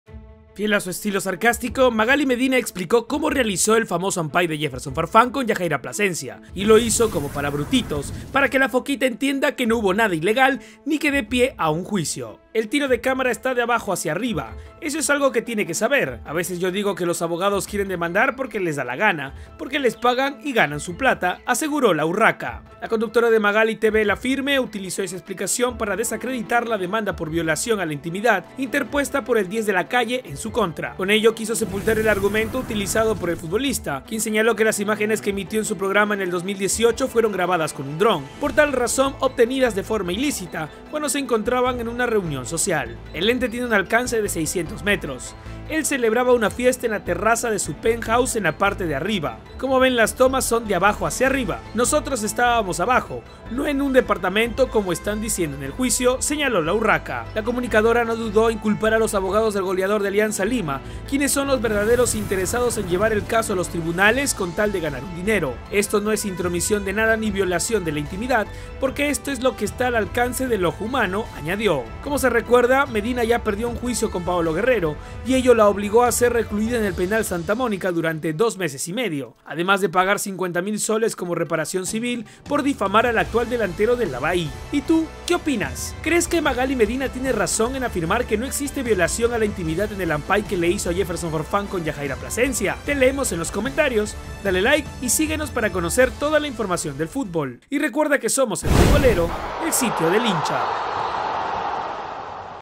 🤣 Magaly Medina tilda de brut0s 🤣 a Yahaira Plasencia y Jefferson Farfán 🇵🇪
MagalyMedina tilda de brut0s a YahairaPlasencia y JeffersonFarfán.